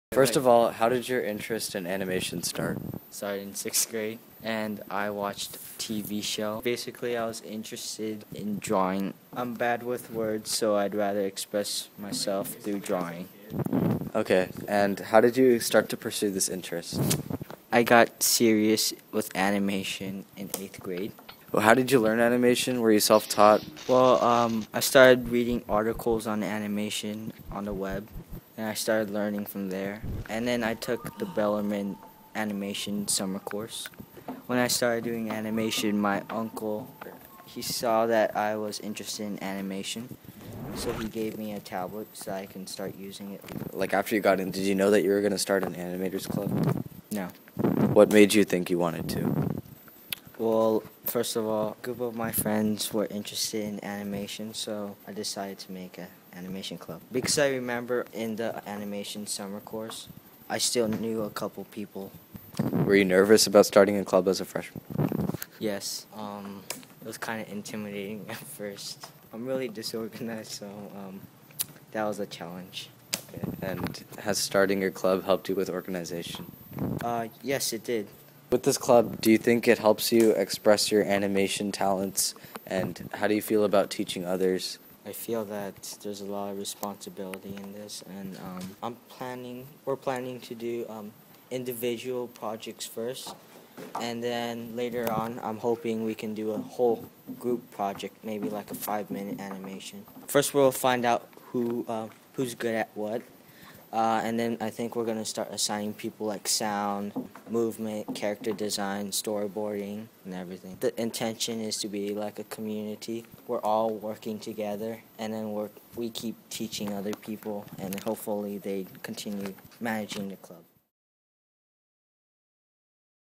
The Animation Station
As the only freshman who manages his own club, Kaito Trias '16 discusses his Animator's Guild, where he teaches members to create animation projects in Adobe Flash using drawing tablets.